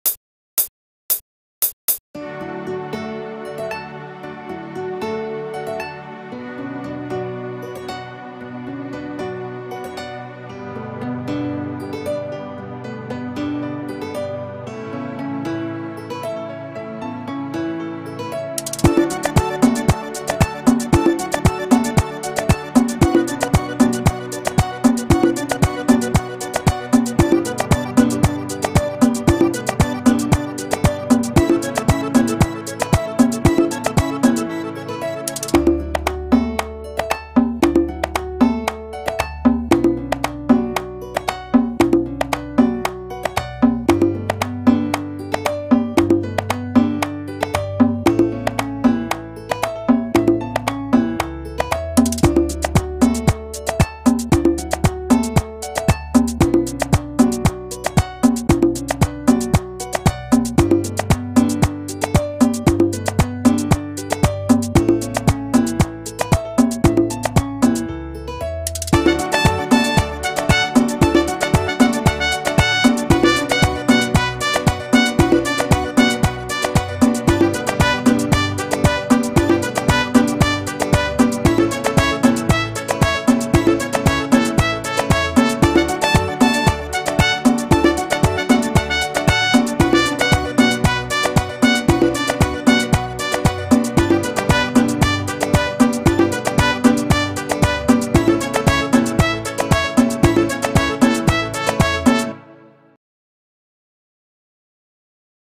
Merengue Romantico FREE --EDIMAR PRODUCIENDO,VCS RECORD.
Este instrumental es de mi autoria esta libre para todo aquel que quiera usarlo, sin mas particular se despide EDIMAR PRODUCIENDO VCS RECORD, bey y tirenme privado si kieren utilizarlo, o comente en esta publicacion.